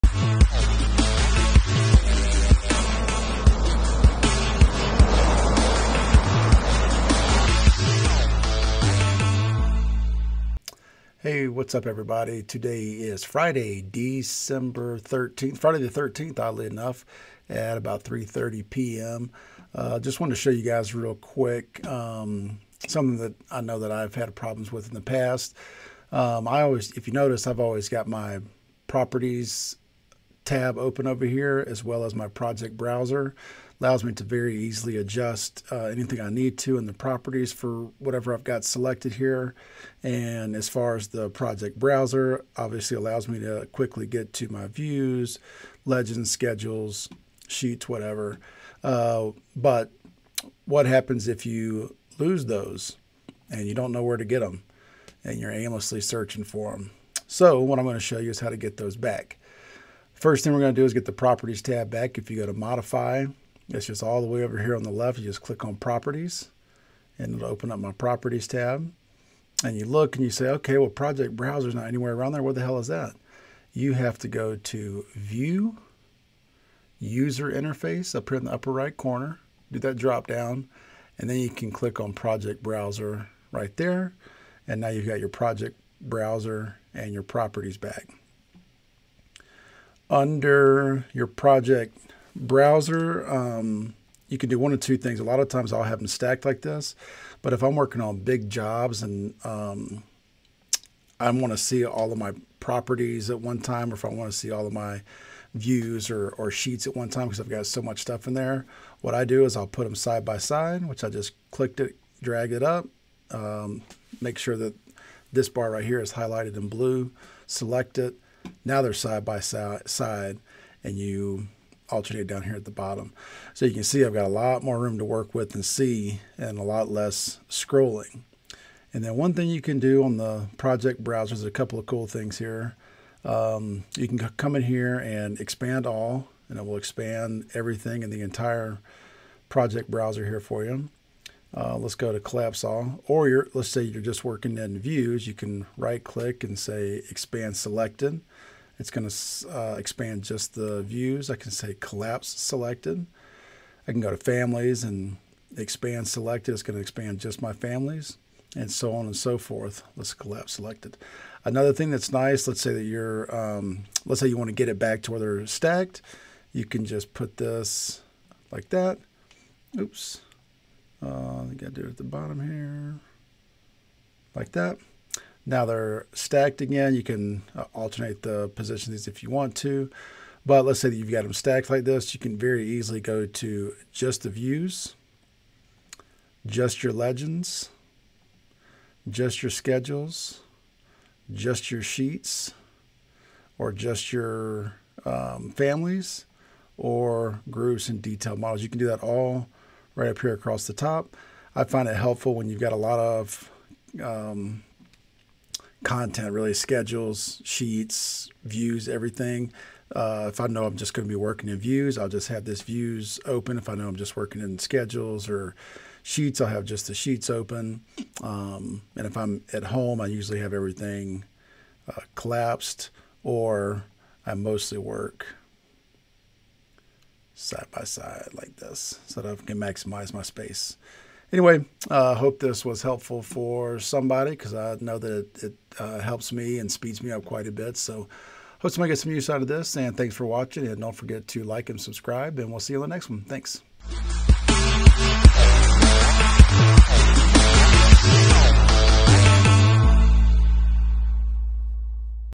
How to Restore the Properties & Project Browser Tabs in Revit
Accidentally closed the Properties or Project Browser tabs in Revit? Learn how to quickly retrieve them and discover useful features in the Project Browser tab that I threw in for free :). This tutorial is perfect for Revit beginners and seasoned users who need a quick fix. Simplify your workflow with these tips!